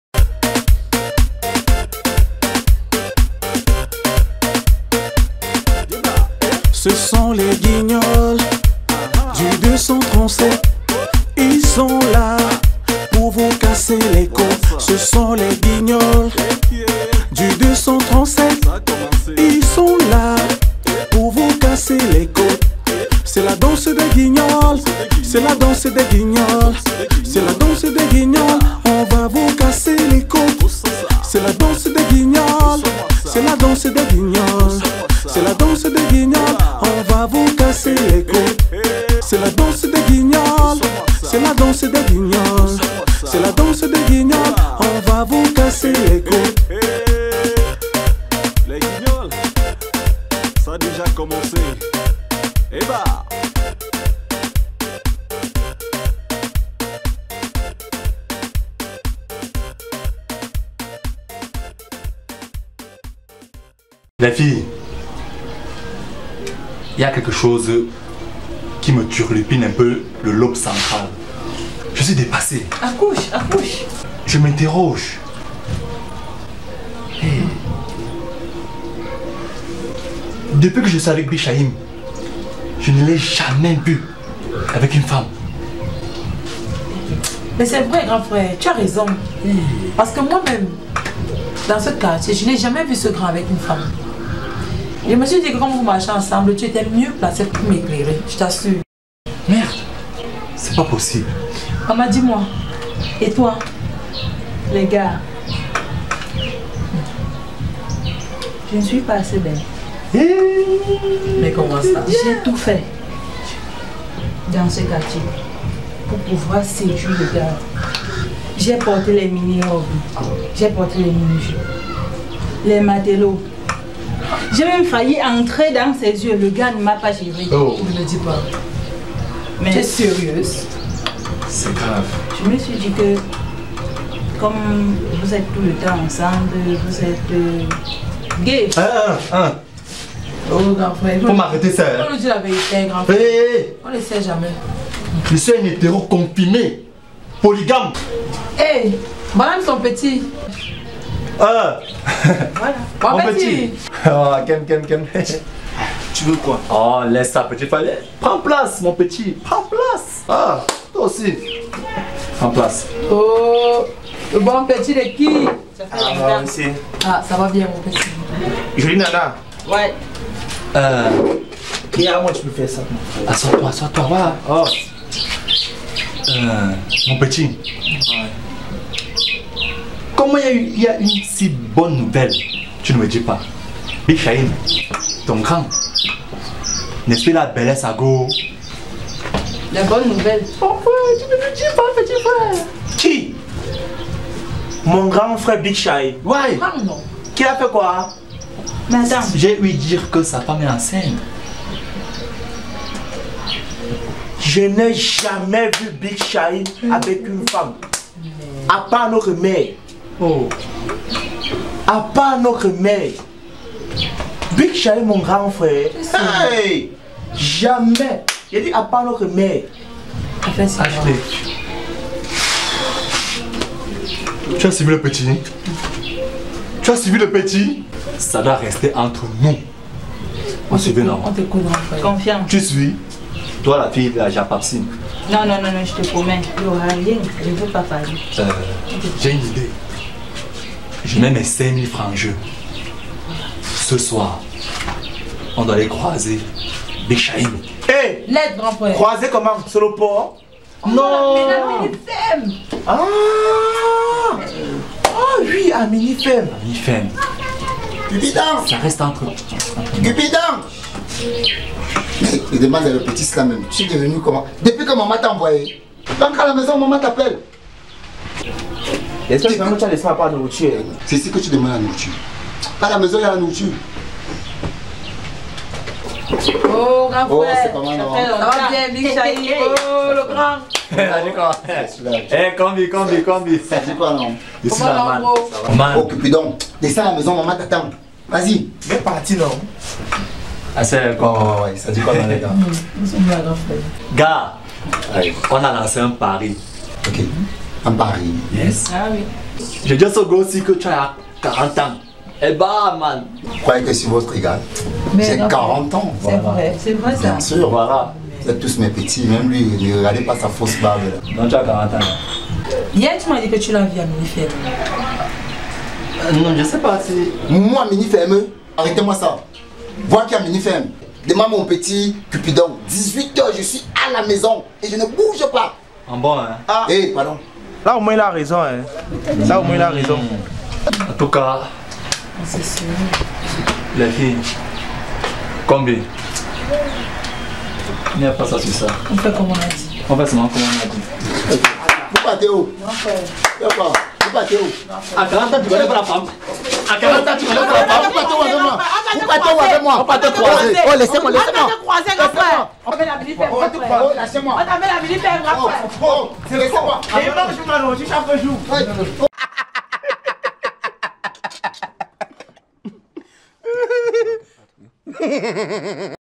40 ans encore puceau ( Les 237 Guignols)
Un groupe d'amis veulent croiser leur camarade qu'ils n'ont jamais vu dans un relation de couple avec une femme

NoLimitProduction